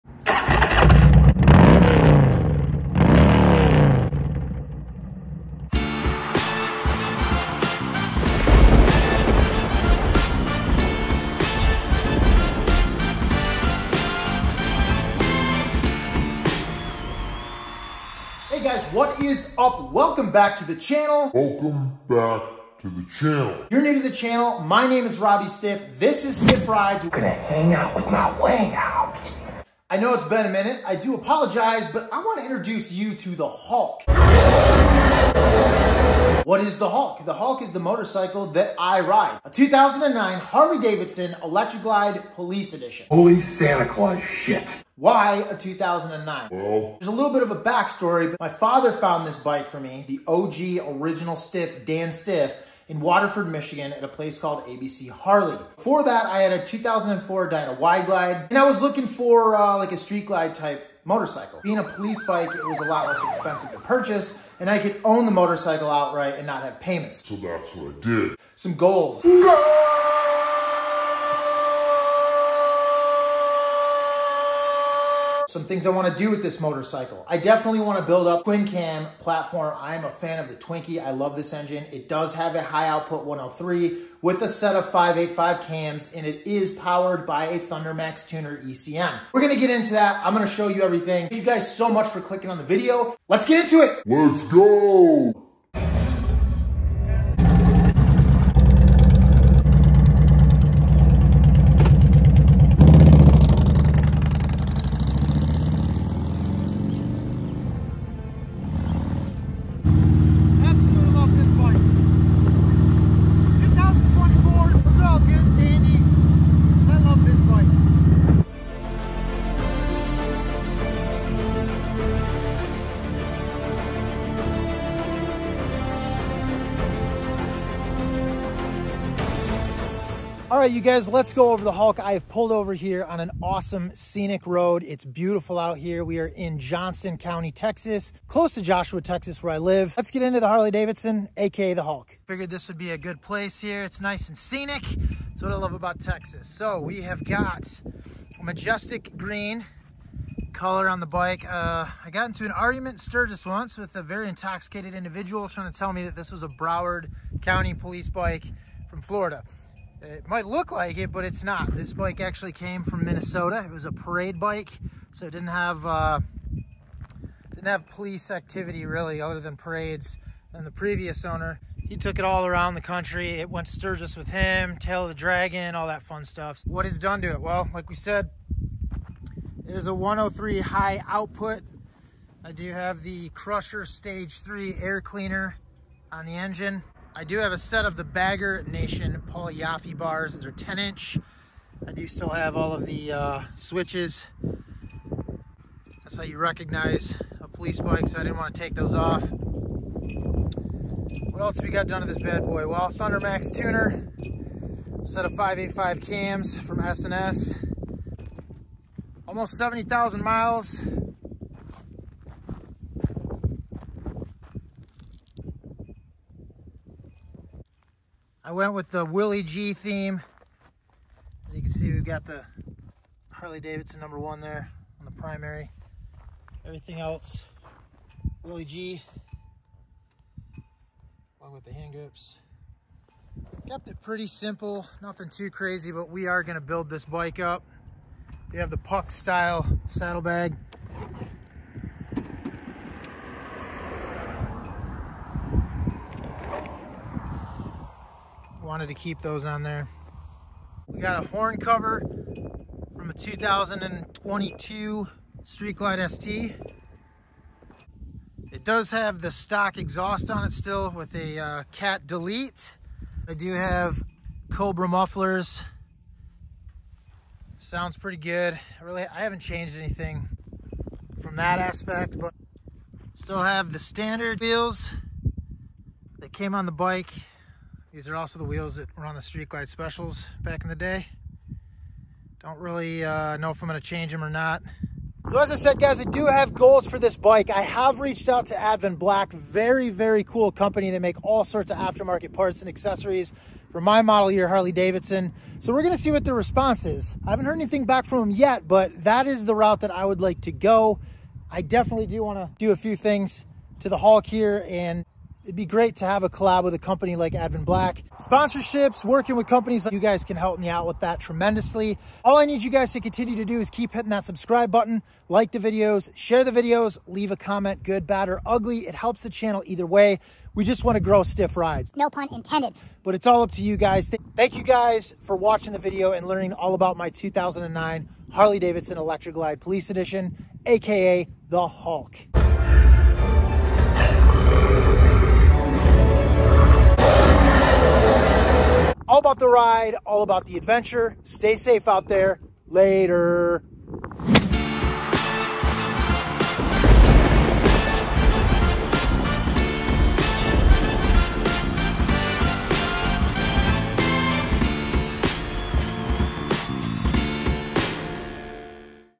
Harley Davidson HULK, 09 Electra Glide Police
This video is all about THE HULK! My 2009 Harley Davidson Electra Glide Police Edition. All of the upgrades parts and a little bit of a background.

Waterfall Style Horn Cover - Gloss Black 61300623 | Harley-Davidson USA

Amazon.com: Paul Yaffe's Bagger Nation 51530512CH Chrome 12" Classic Bagger Apes Handlebar (for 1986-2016 Harley-Davidson FLH Touring Models, 1") : Automotive

Crusher Street Sleeper III Air Cleaner Kit - 9606 - Dennis Kirk

ThunderMax for 08-13 Touring® Models

Official Harley-Davidson® dealer | Texas Harley-Davidson®